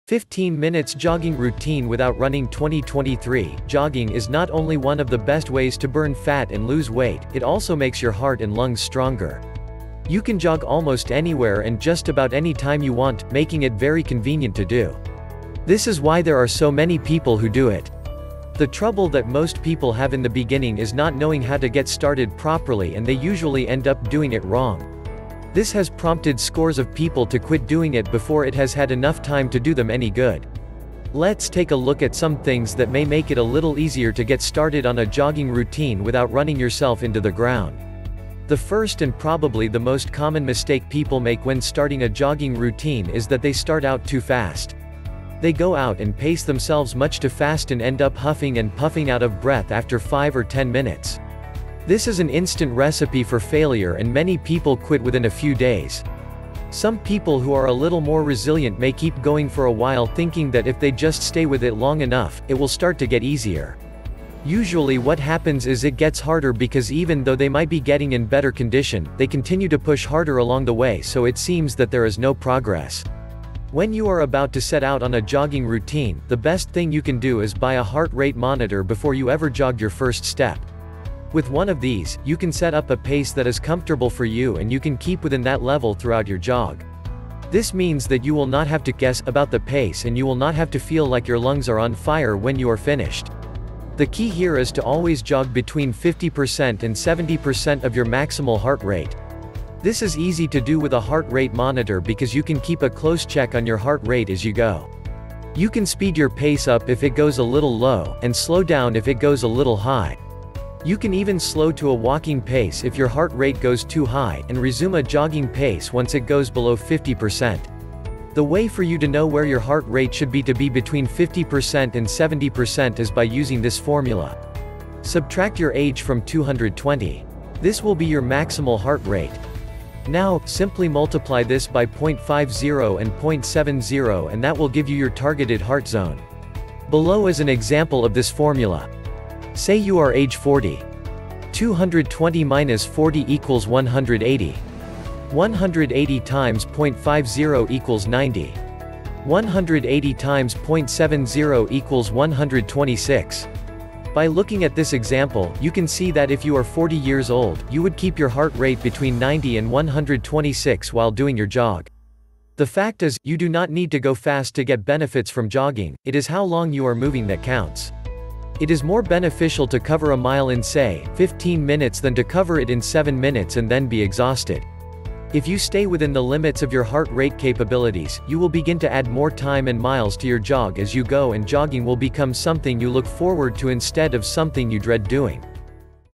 15 Minutes Jogging Routine Without Running 2023
15 Minutes Jogging Routine Without Running 2023.Jogging is not only one of the best ways to burn fat and lose weight; it also makes your heart and lungs stronger. You can jog almost anywhere and just about any time you want, making it very convenient to do. This is why there are so many people who do it.

The trouble that most people have in the beginning is not knowing how to get started properly and they usually end up doing it wrong. This has prompted scores of people to quit doing it before it has had enough time to do them any good.

Let’s take a look at some things that may make it a little easier to get started on a jogging routine without running yourself into the ground.

The first and probably the most common mistake people make when starting a jogging routine is that they start out too fast. They go out and pace themselves much to fast and end up huffing and puffing out of breath after 5 or 10 minutes. This is an instant recipe for failure and many people quit within a few days.

Some people who are a little more resilient may keep going for a while thinking that if they just stay with it long enough, it will start to get easier. Usually what happens is it gets harder because even though they might be getting in better condition, they continue to push harder along the way so it seems that there is no progress.

When you are about to set out on a jogging routine, the best thing you can do is buy a heart rate monitor before you ever jog your first step. With one of these, you can set up a pace that is comfortable for you and you can keep within that level throughout your jog. This means that you will not have to “guess” about the pace and you will not have to feel like your lungs are on fire when you are finished.

The key here is to always jog between 50% and 70% of your maximal heart rate. This is easy to do with a heart rate monitor because you can keep a close check on your heart rate as you go. You can speed your pace up if it goes a little low, and slow down if it goes a little high. You can even slow to a walking pace if your heart rate goes too high, and resume a jogging pace once it goes below 50%.

The way for you to know where your heart rate should be to be between 50% and 70% is by using this formula:

Subtract your age from 220. This will be your maximal heart rate. Now, simply multiply this by .50 and .70 and that will give you your targeted heart zone. Below is an example of this formula.

Say you are age 40

220 minus 40 equals 180

180 times .50 equals 90

180 times .70 equals 126

By looking at this example, you can see that if you are 40 years old, you would keep your heart rate between 90 and 126 while doing your jog.

The fact is, you do not need to go fast to get benefits from jogging, it is how long you are moving that counts. It is more beneficial to cover a mile in say, 15 minutes than to cover it in 7 minutes and then be exhausted.

If you stay within the limits of your heart rate capabilities, you will begin to add more time and miles to your jog as you go and jogging will become something you look forward to instead of something you dread doing.